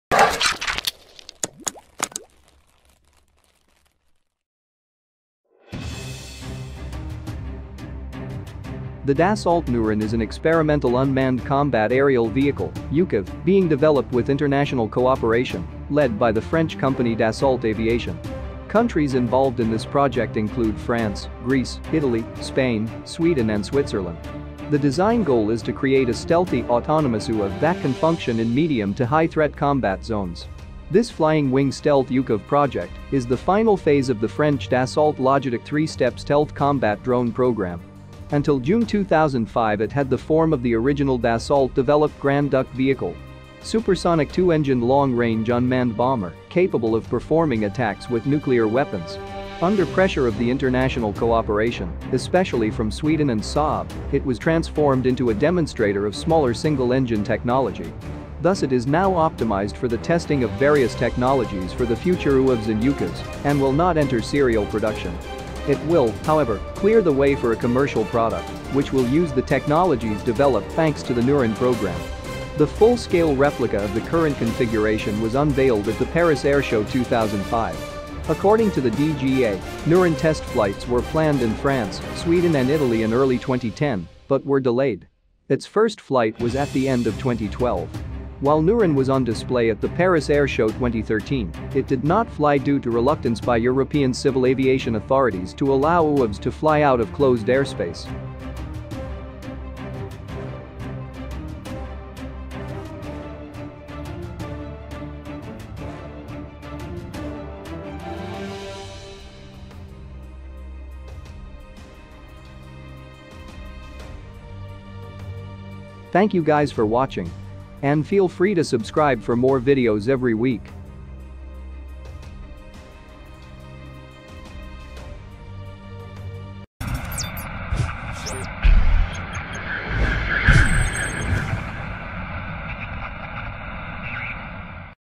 Dassault nEUROn, Designed With Stealth Technology
The design goal is to create a stealthy, autonomous UAV that can function in medium- to high-threat combat zones.

This flying wing stealth UCAV project is the final phase of the French Dassault LOGIDUC 3-step stealth "combat drone" programme. Until June 2005 it had the form of the original Dassault developed Grand Duc vehicle: supersonic two-engined long-range unmanned bomber, capable of performing attacks with nuclear weapons.